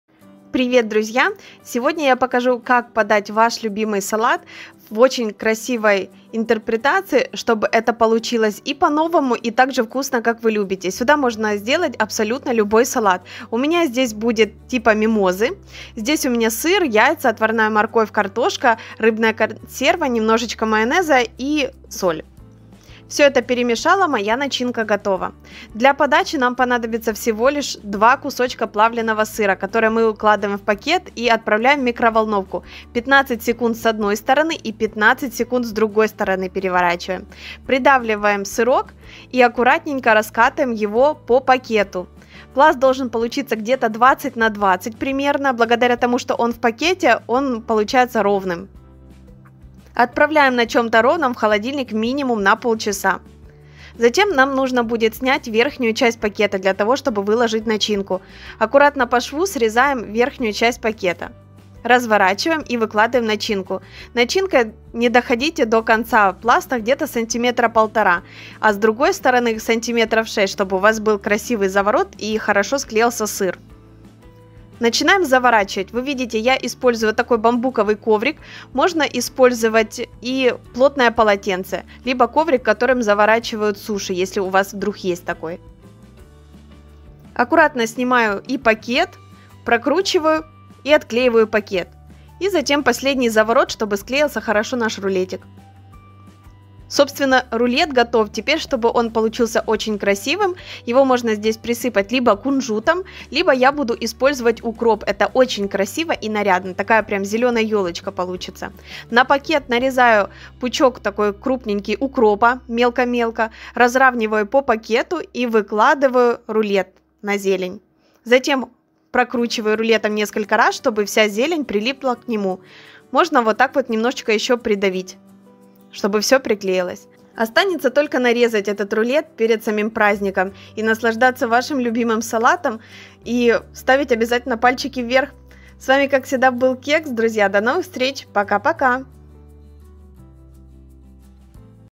БЕРУ 2 СЫРКА и НАГРЕВАЮ В ПАКЕТЕ.Удивлю Гостей.I TAKE 2 CHEESE and Salad Surprised the guests.Кексик
Огромный плюс в том,что можно завернуть все что хотите.
Я делаю с жаренными грибами и с кальмаром+сыр+яйца

А в этот раз у меня "Мимоза":
1 рыбная консерва в масле
1 крупная вареная картофелина
1 крупная морковь
2-3 яйца
70-100г твердого сыра
соль,майонез по вкусу

Для самого рулета:
 2 плавленых сырка
пучок укропа

A huge plus is that you can wrap whatever you want.
I make with fried mushrooms and squid + cheese + eggs

And this time I have "Mimosa":
1 canned fish in oil
1 large boiled potato
1 large carrot
2-3 eggs
70-100g hard cheese
salt, mayonnaise to taste

For the roll itself:
  2 processed cheese
a bunch of dill